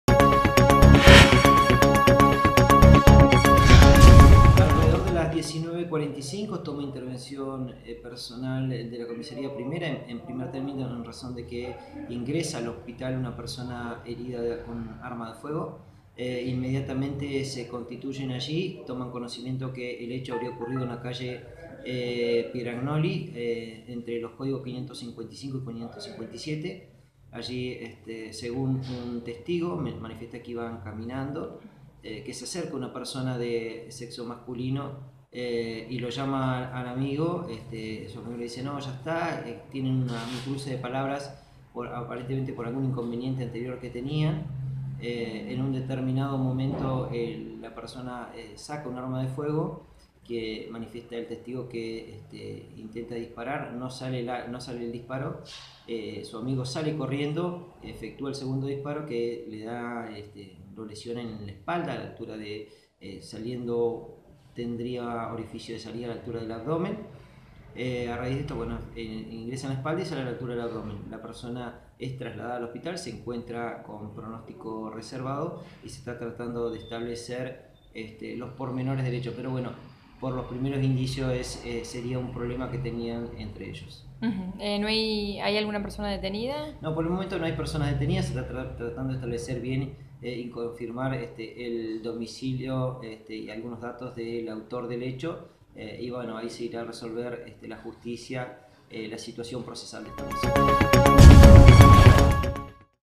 BALEAN A UN HOMBRE EN LA CALLE. ESTA GRAVE.
Ocurrió en calle Pieragnoli.
César Brandt / Jefe de Operaciones de la Policía